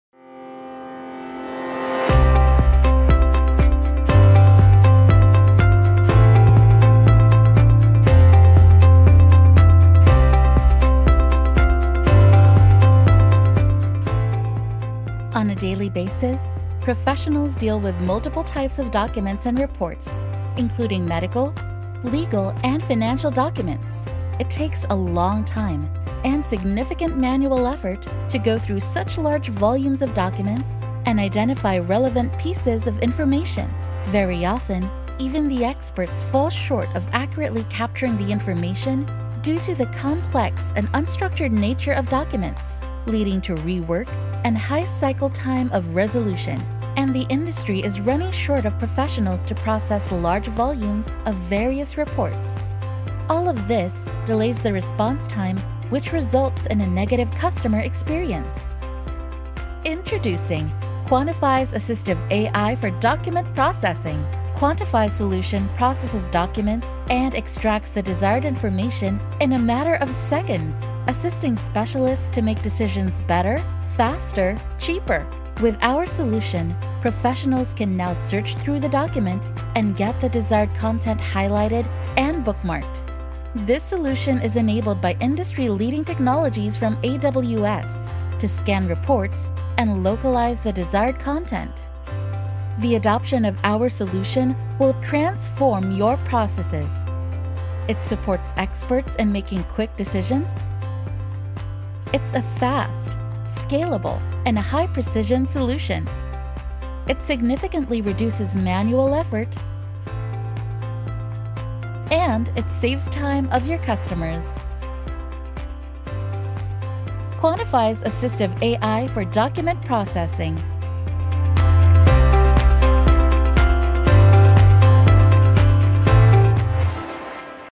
Document Processing Solution
Quantiphi’s end to end cognitive document processing solution built on Amazon Textract is a deep learning-based, template-free optical character recognition (OCR) and natural language processing (NLP) solution that covers the complete lifecycle of document processing.